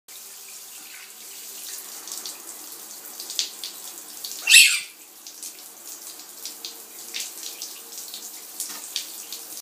Screaming Rabbit
My rabbit Carbon is screaming while I wash her!